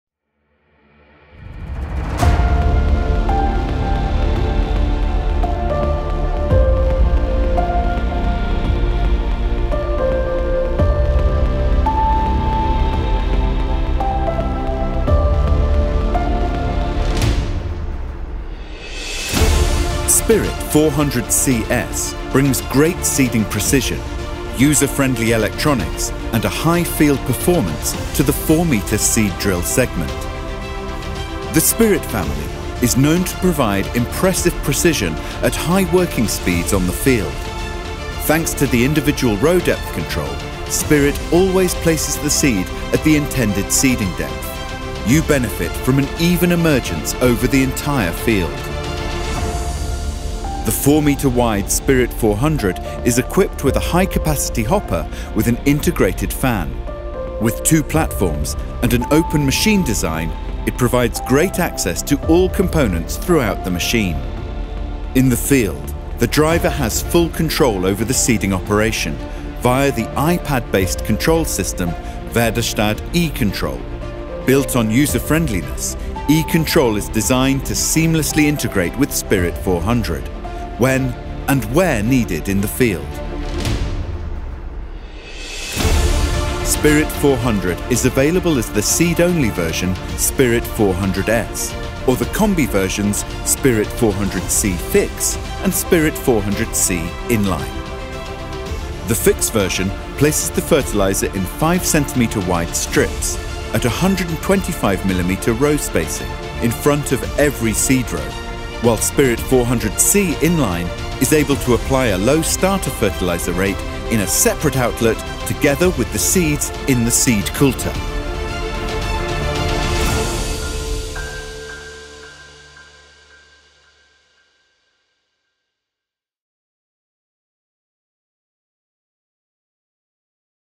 Väderstad Spirit 400C/S
The pneumatic seed drill Väderstad Spirit 400C/S brings great seeding precision, user-friendly electronics and a high field performance to the 4-metre seed drill segment. The Spirit family is known to provide impressive precision at high working speeds on the field.

Learn more about how the new Spirit 400C/S will provide a great start for you crops